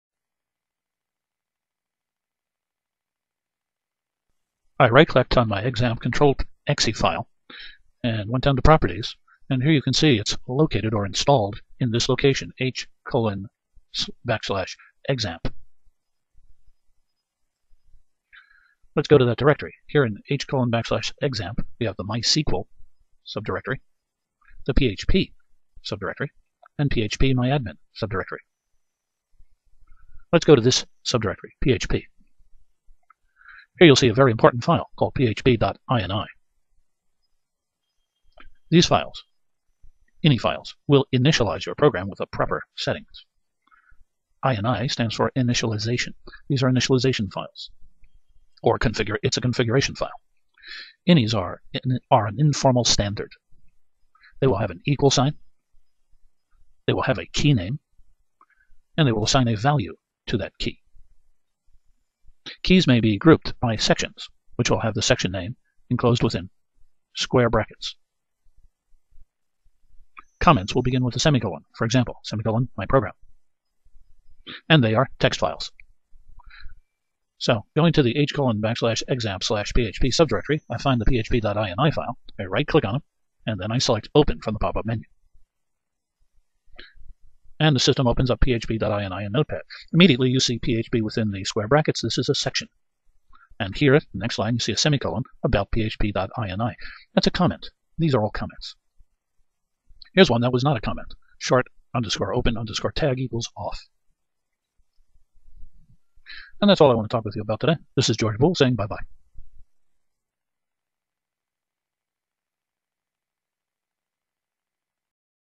Xampp directory, php folder, and php.ini
A look at the Xampp directory structure and some of the files
therein.